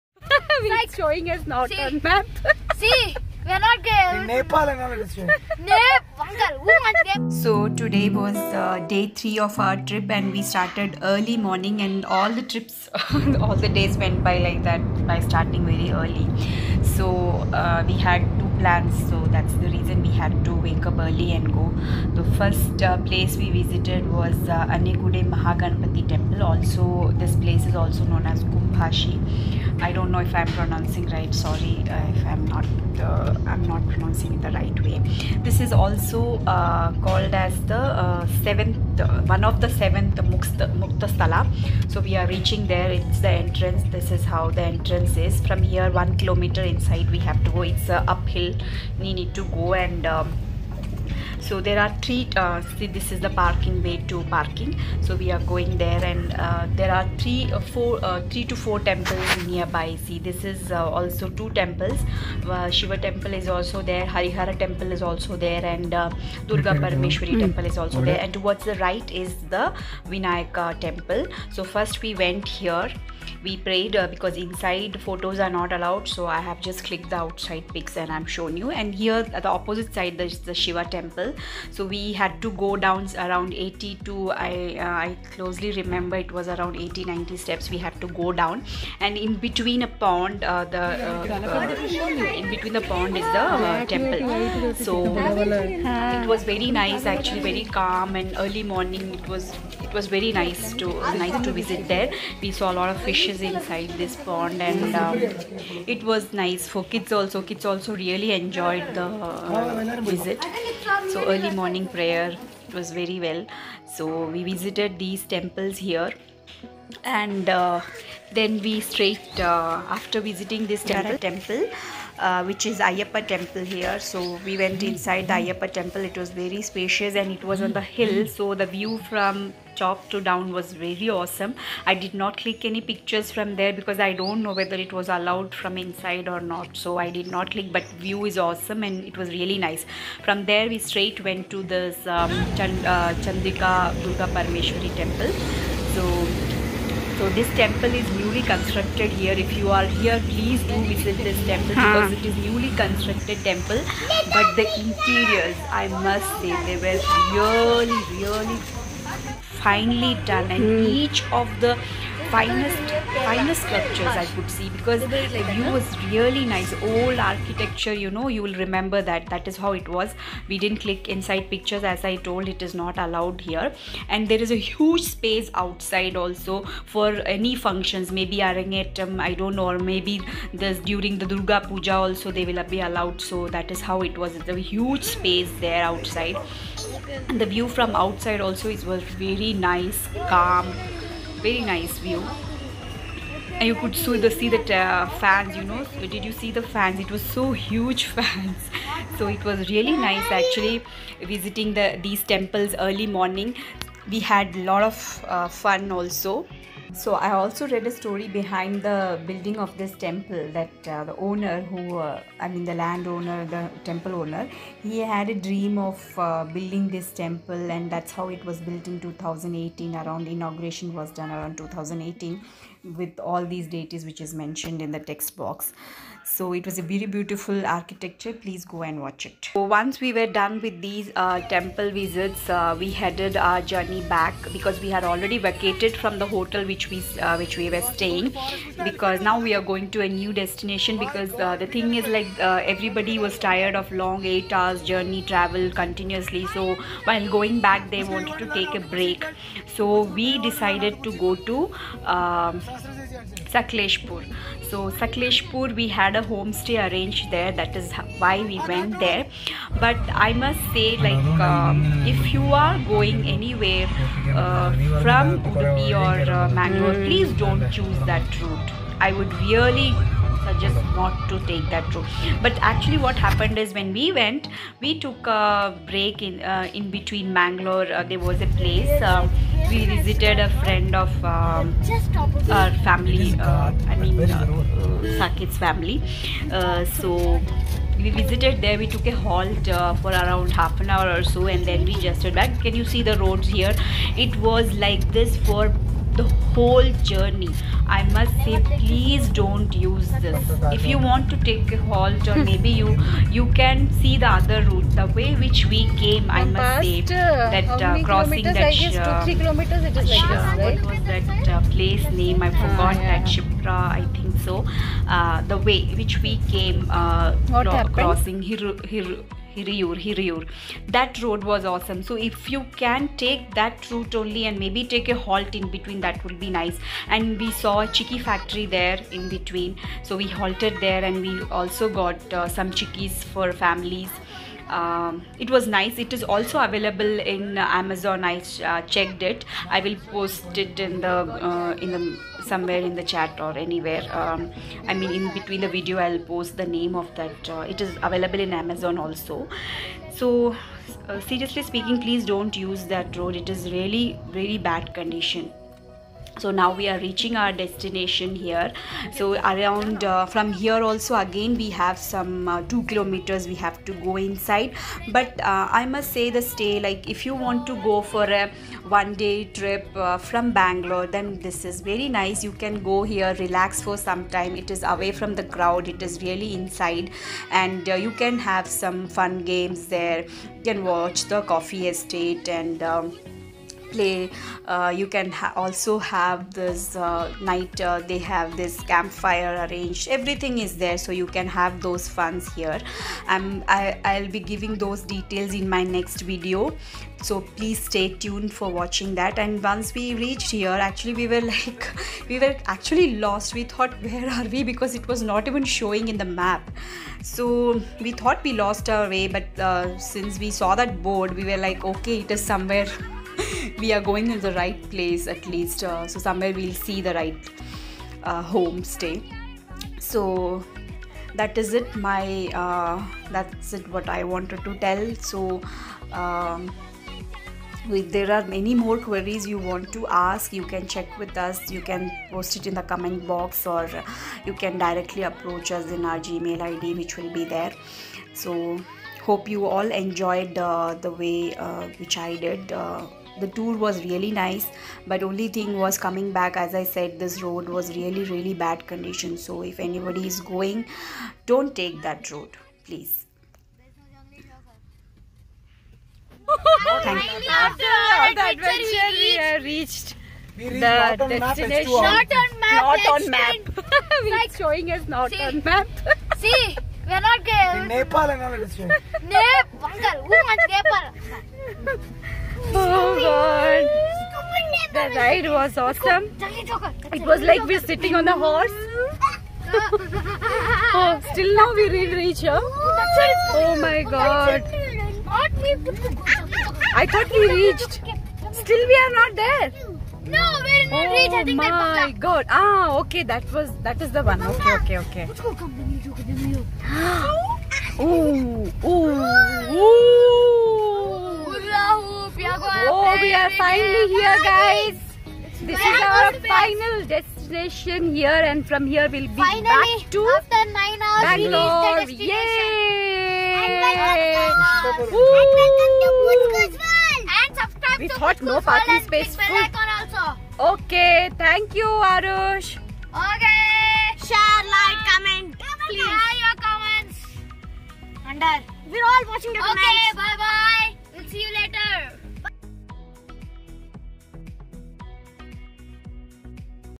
Visit to kumbhashi vinayaka temple ,chandika durga parmeshwari temple and a tiring drive
Visit to kumbhashi and tiring  drive